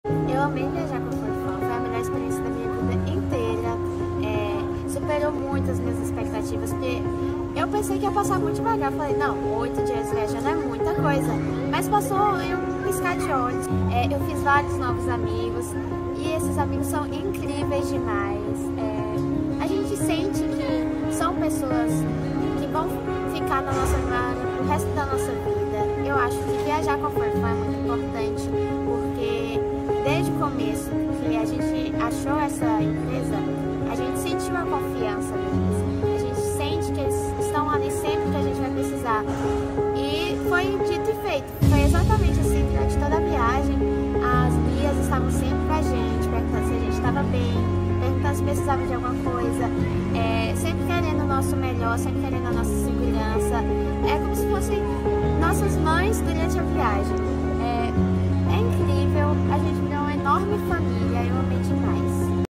Depoimento Manu - BA - Temporada EuroFun 2023
A viagem da vida pelos olhos da passageira Manu de Tanque Novo - BA.